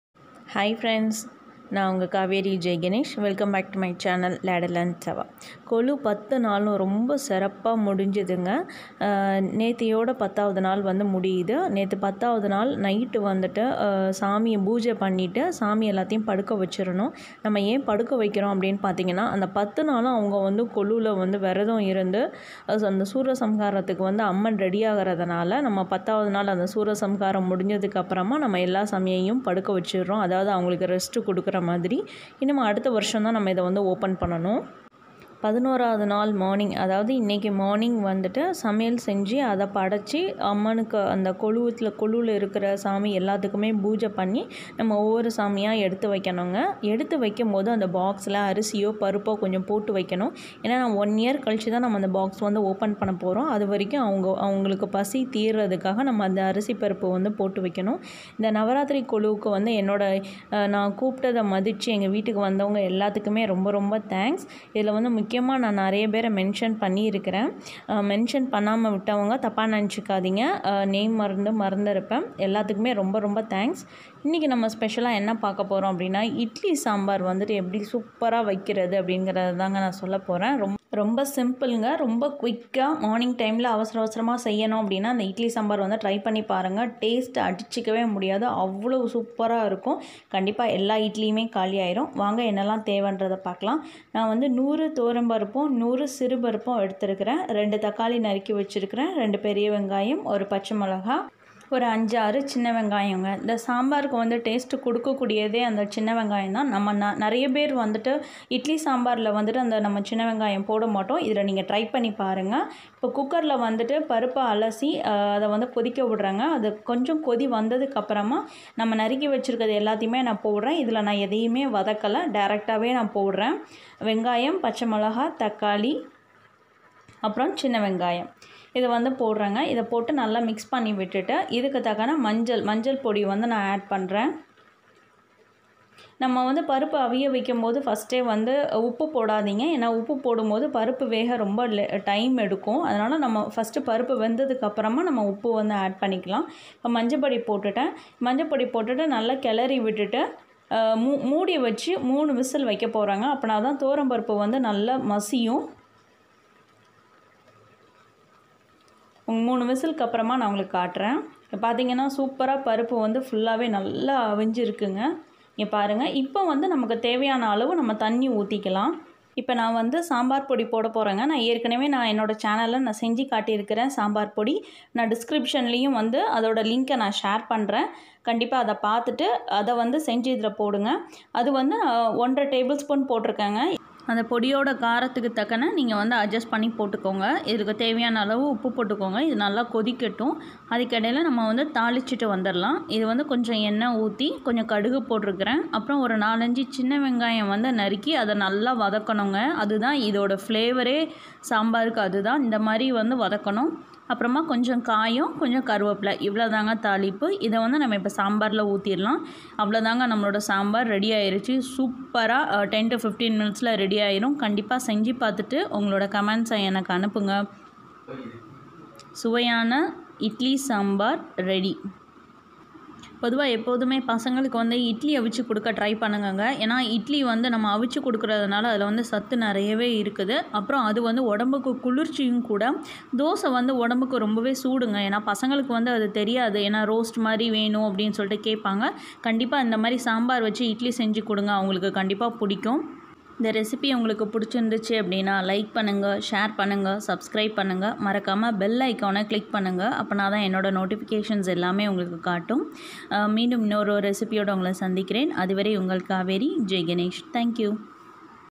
சுவையான இட்லி சாம்பார்| கொலு 10 நாள் கொண்டாட்டம் | Ladle And Tawa | KAVERI JAIGANESH
Ingredients:-
துவரம் பருப்பு-100gm
சிறு பருப்பு -100gm
Onion-2
Tomato-2
Green chilli-1
Small onion-6
Sambar podi- 1 1/2 tbspn
Salt

Mustard-1/2 tspn
Small onion-4
Kayam
Curry leaves